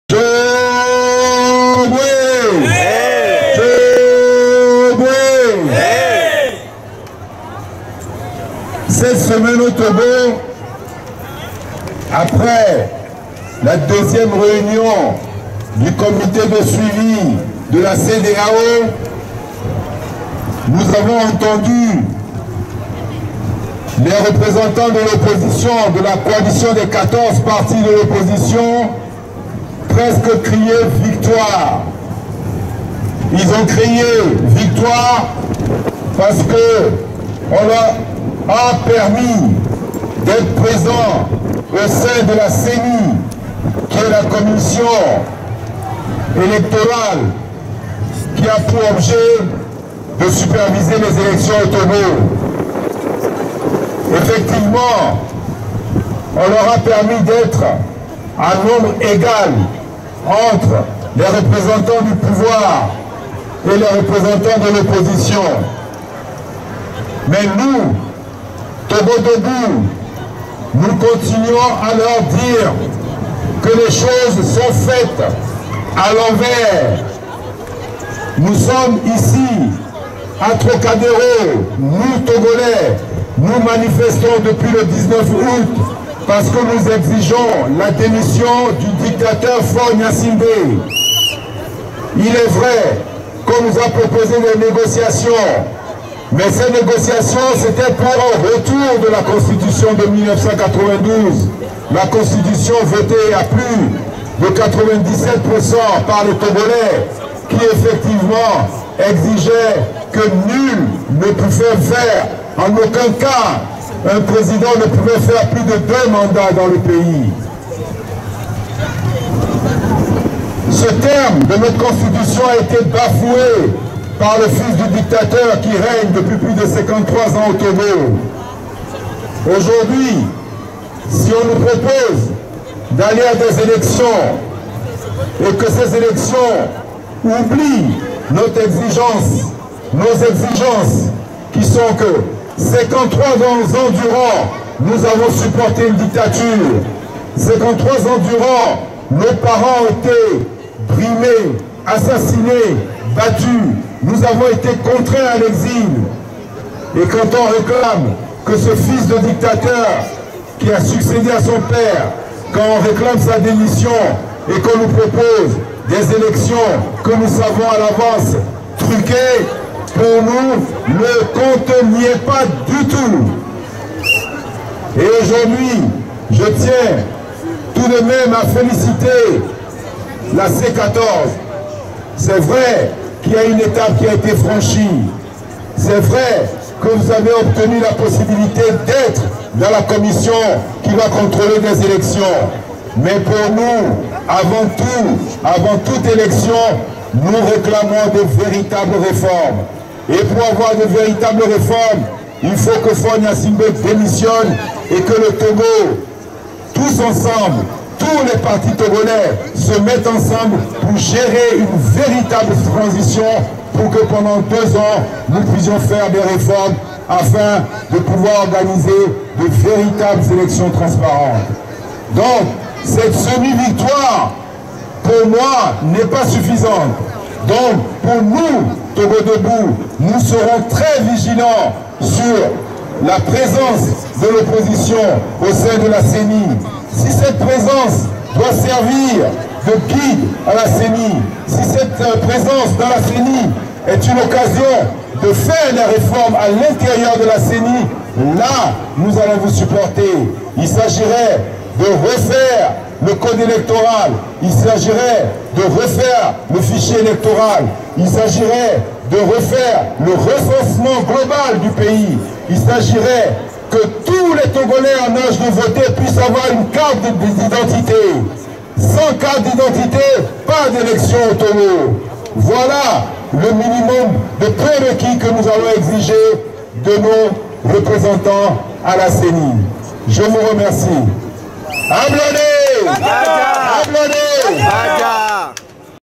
TogoDebout Avant toutes élections, nous réclamons de véritables réformes
Samedi 29 Septembre 2018 - Sit-in hebdomadaire Trocadéro
TogoDebout félicite la C14 pour sa détermination mais exige de véritables réformes institutionnelles et constitutionnelles avant toutes élections au Togo